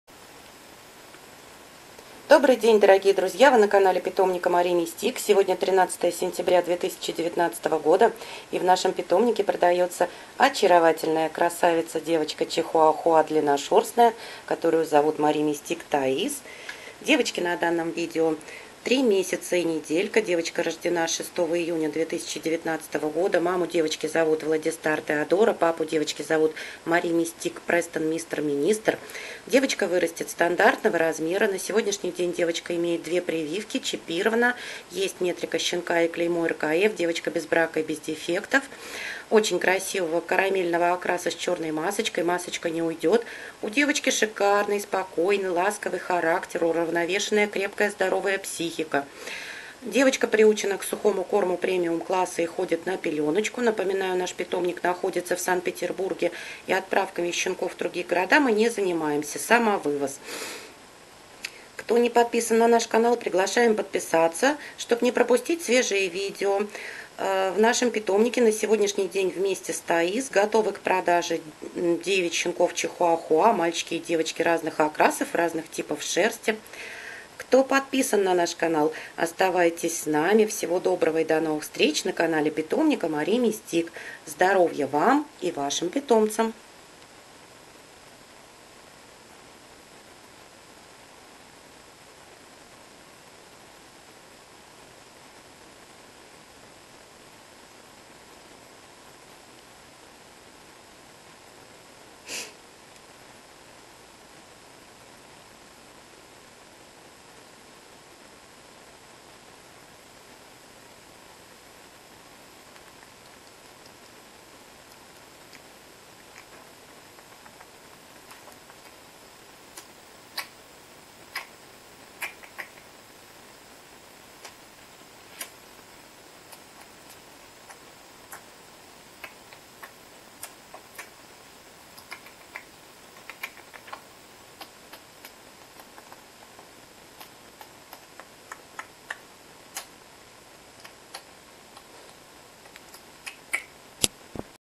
ПРОДАЮ - МАРИ МИСТИК ТАИС - 3 МЕС 1 НЕД
САНКТ - ПЕТЕРБУРГ . ПРОДАЮТСЯ
9 ЩЕНКОВ ЧИХУАХУА ИЗ ПИТОМНИКА РКФ " МАРИ МИСТИК "
Д Р ИЮНЬ 2019 - ЕСТЬ МИНИ , МЕЛКИЙ СТАНДАРТ , СТАНДАРТ . МАЛЬЧИКИ И ДЕВОЧКИ , САМЫХ РАЗНЫХ ОКРАСОВ . ПОЛНОСТЬЮ ПРИВИТЫ , ЧИПИРОВАНЫ , ДОКУМЕНТЫ И КЛЕЙМА РКФ 💞💞💞💞💞💞💞💞💞💞💞💞💞💞💞💞💞💞💞💞💞💞МНОГО ФОТО И ВИДЕО ВСЕХ ЩЕНКОВ НА ПРОДАЖУ И ИХ РОДИТЕЛЕЙ МОЖНО ПОСМОТРЕТЬ НА МОЕЙ СТР. В ВК И ВИДЕО В ЮТУБЕ